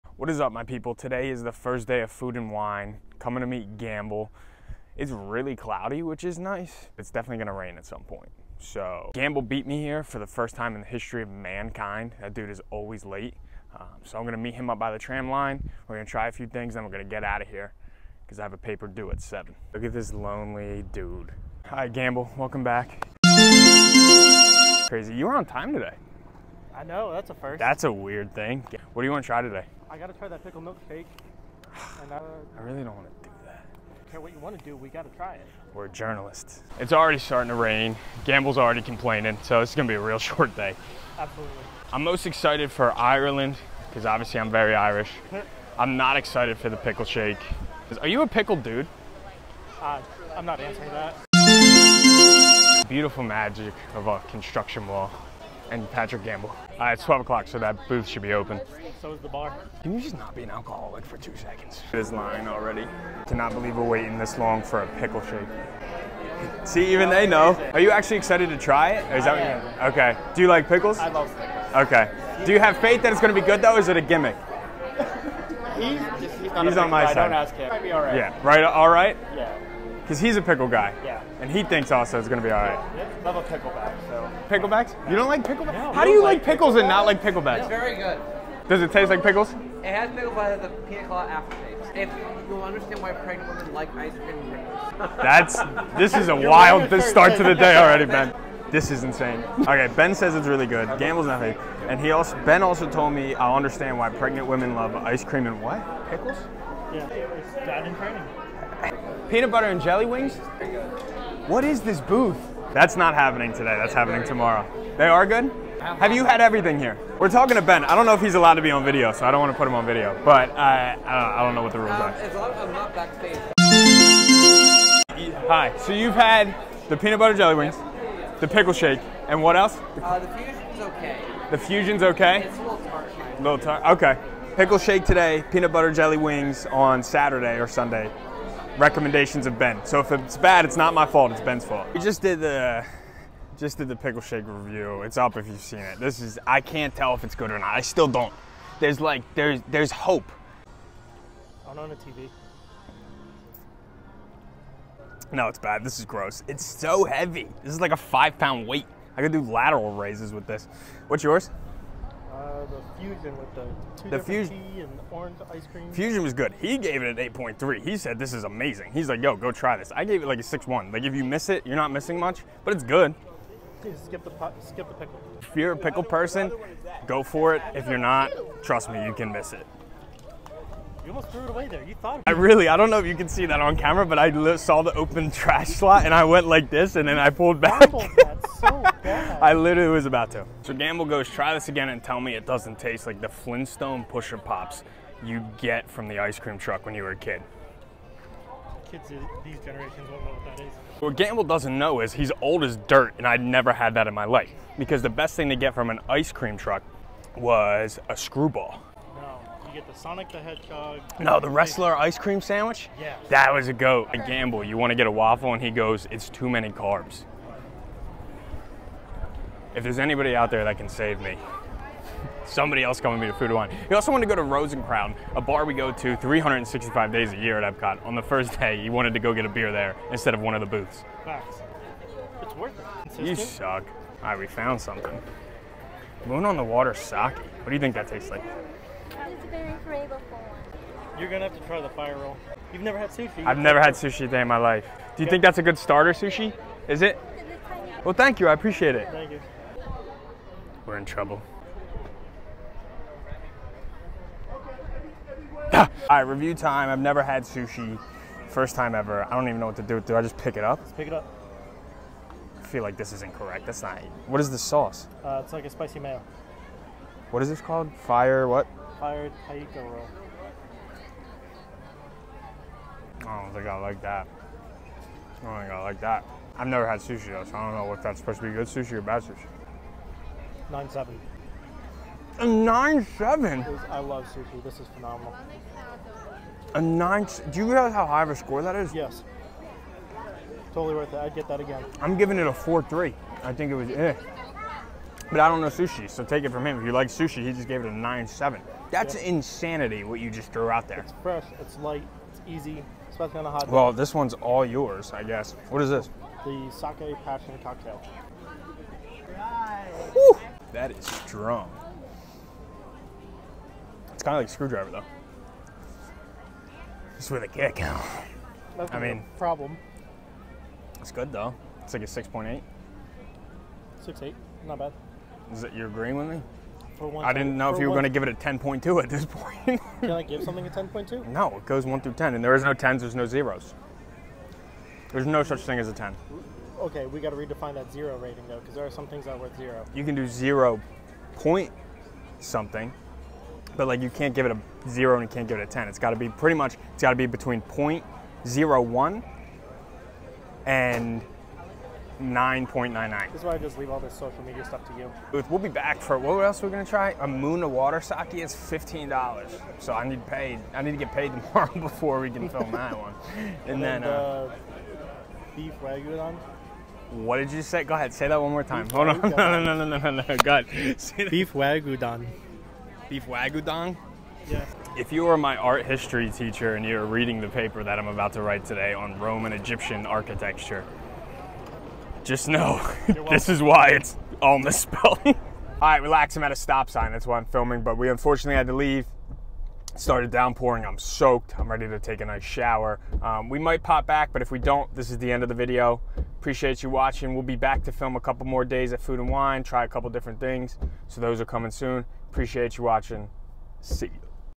Pickle Shakes & More | Food & Wine 2023 Day 1
Day One of Food & Wine 2023! Pickle and Fusion Shakes. Trying sushi for the first time. Gamble throws out some ridiculous scores.